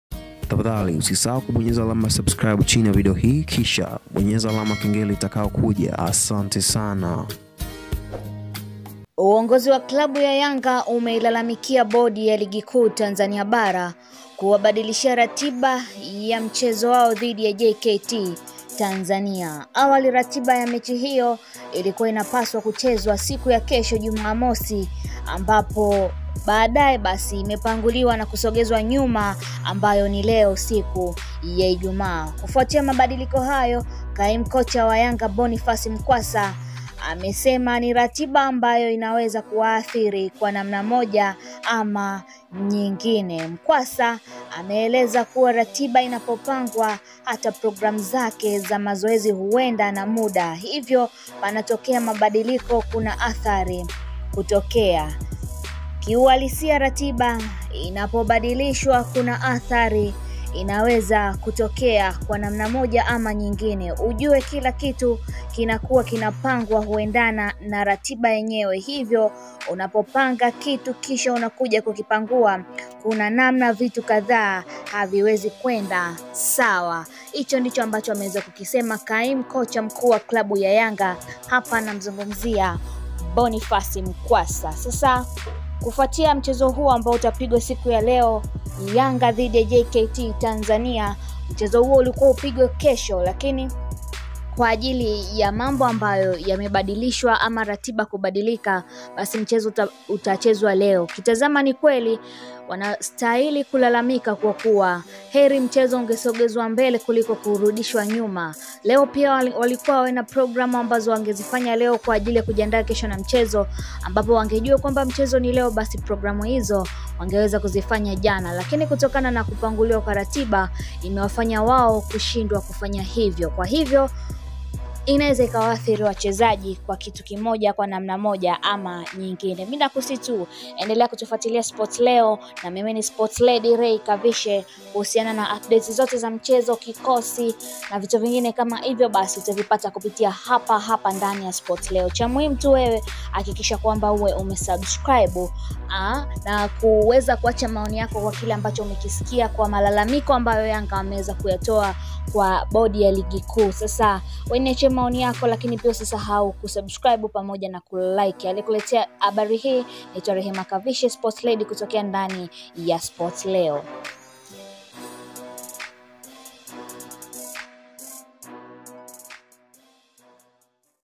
YANGA WAISHANGAA BODI YA LIGI KISA....
Uongozi wa klabu ya Yanga umeilalamikia bodi ya ligi kuwabadilishia ratiba ya mchezo wao dhidi ya JKT Tanzania.

Awali ratiba ya mechi hiyo ilikuwa inapaswa kuchezwa Jumamosi ya wiki hii lakini baadaye ikapanguliwa na kusogezwa nyuma ambayo ni leo Ijumaa.

Kufuatia mabadiliko hayo, Kaimu Kocha wa Yanga, Boniface Mkwasa, amesema ni ratiba ambayo inaweza kuwaathiri kwa namna moja ama nyingine.

Mkwasa ameeleza ratina inapopangwa hata program zake za mazoezi huenda na muda hivyo panatokea mabadiliko kuna athari hutokea.

"Kiuhalisia ratiba inapobadilishwa kuna athari inaweza kutokea kwa namna moja ama nyingine.

"Ujue kila kitu kinakuwa kinapangwa kuendana na ratiba yenyewe, hivyo unapopanga kitu kisha unakuja kukipangua kuna namna vitu kadhaa haviwezi kwenda sawa."